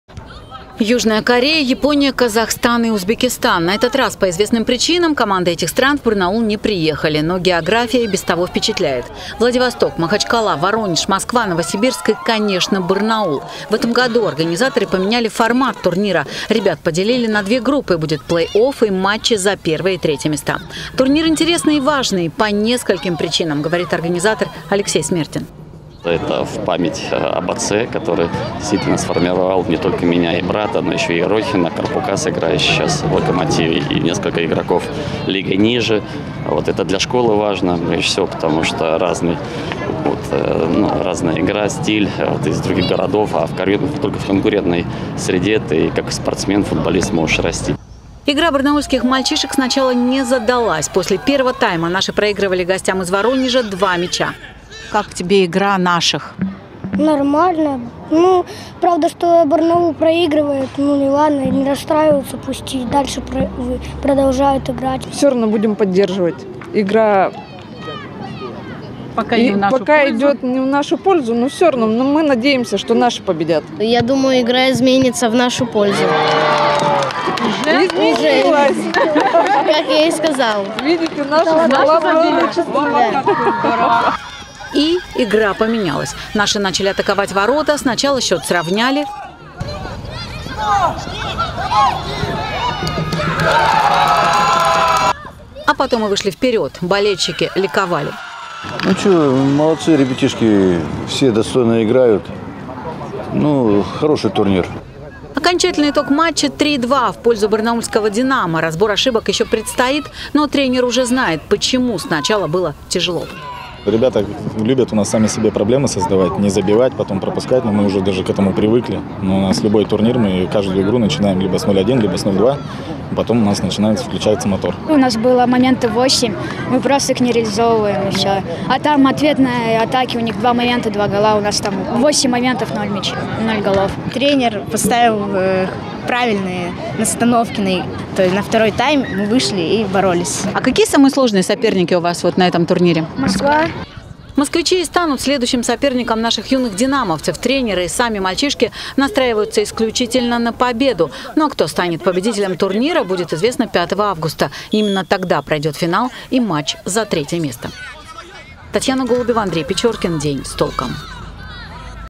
В Барнауле открылся детский турнир по футболу памяти Геннадия Смертина
Шестой по счету, но в новом формате. В Барнауле открылся детский турнир по футболу памяти Геннадия Смертина. Больше сотни футболистов 2011 года рождения,  от Москвы до Владивостока, будут бороться за победу. Сегодня в первом матче гостей из Воронежа принимали мальчишки барнаульского «Динамо»